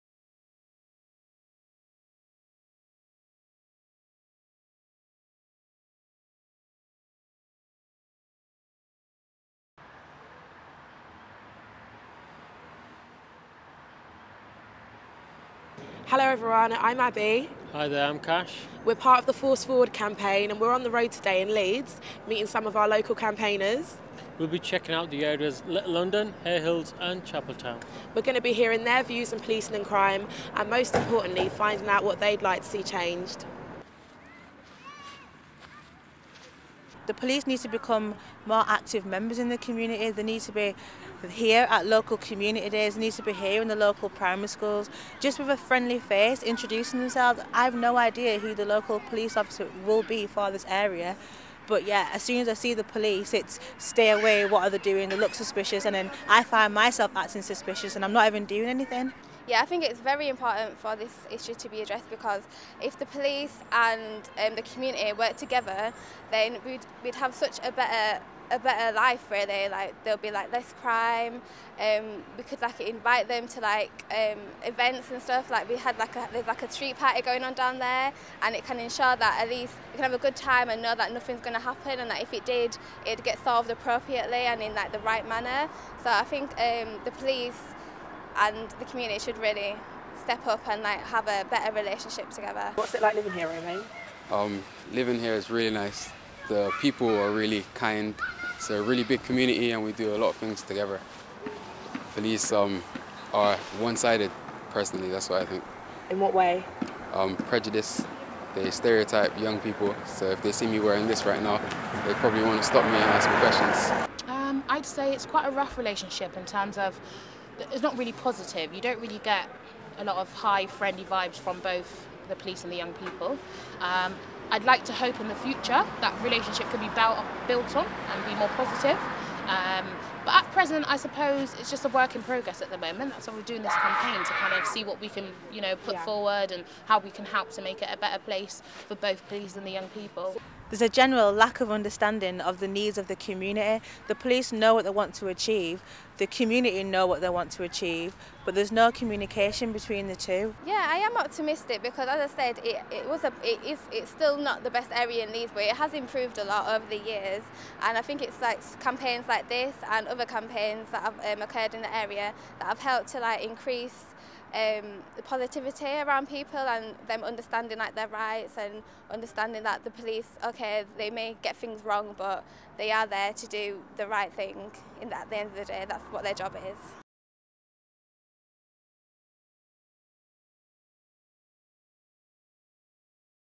Police Crime Commissioner Campaign
ForceForward is giving young people a chance to change and shape the way policing is carried out in their areas and across England, that will have a positive impact in the Future.

The the vote will be held on November 15th 2012 - Sign up to vote, Make a Difference!